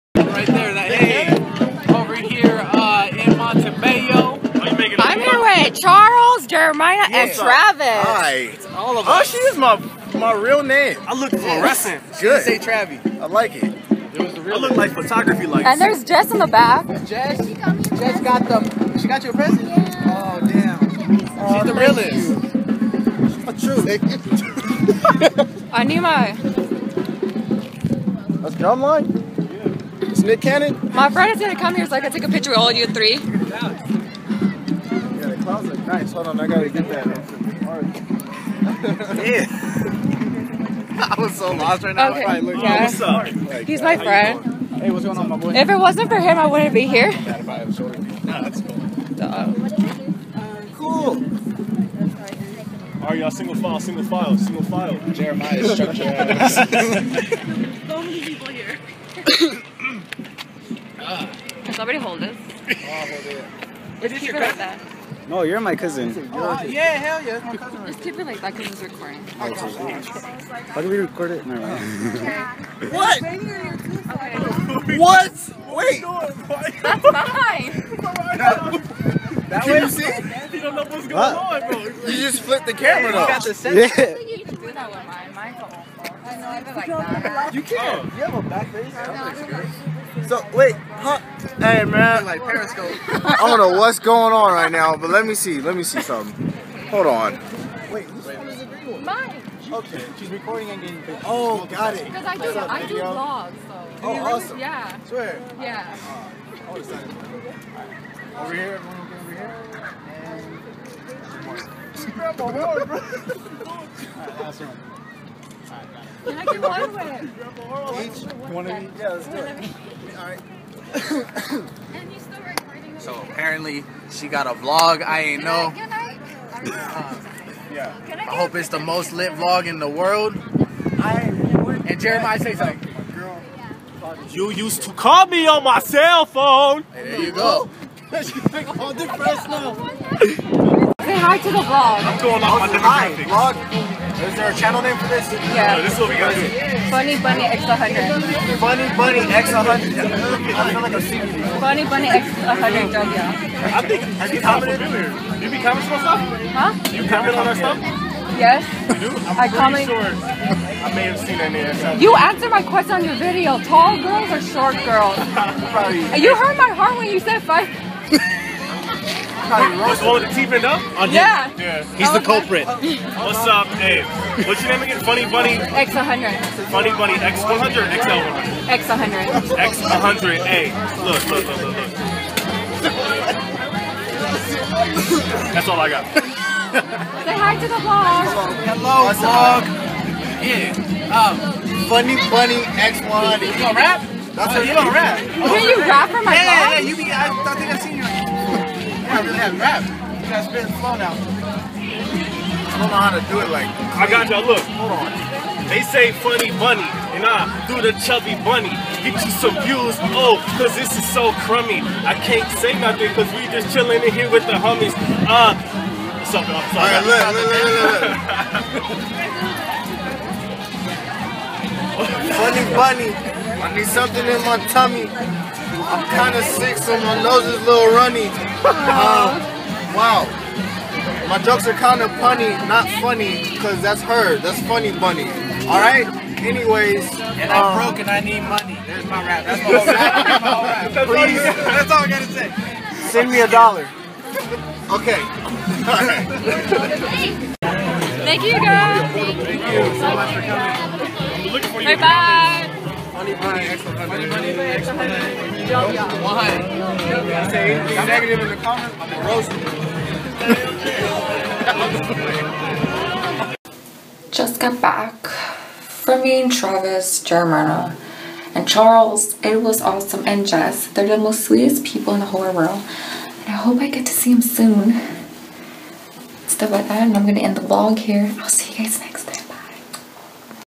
The Rap Gang Vlog
Hey everyone its me Tatiana. In this vlog I got to meet some awesome people at a cancer walk today. It was really cold and windy, but still a good day. Hope you like!

I love you all my funnybunnies!!